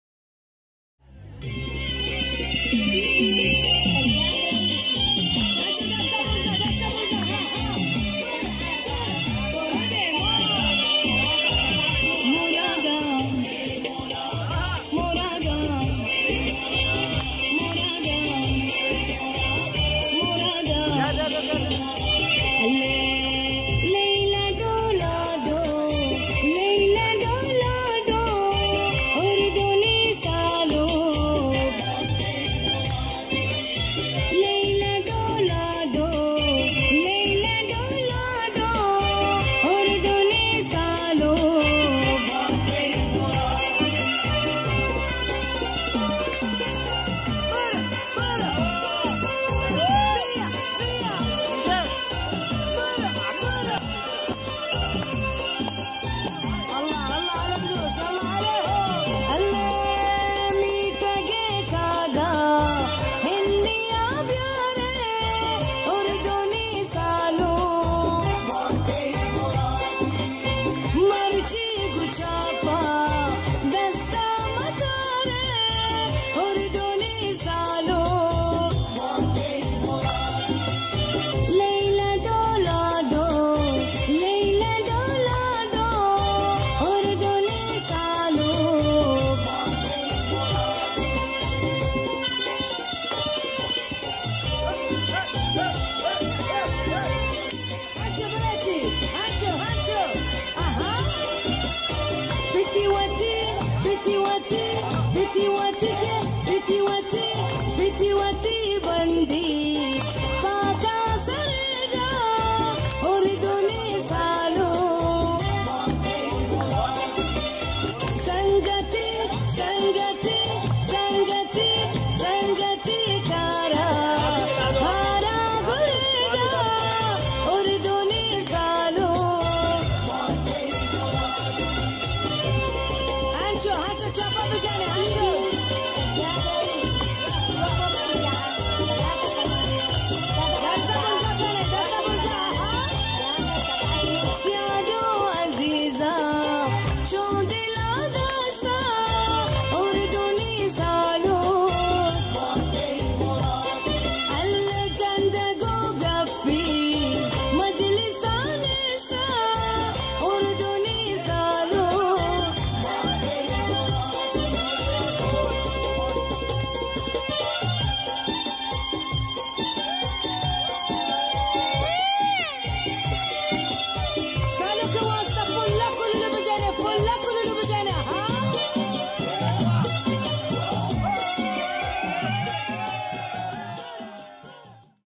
Dochapi Nach Shaheed Hayat Baloch With Friends
Shaheed Hayat Baloch and his Friend Visited IBA university Karachi Program
and Perform Dochapi Nach With Friends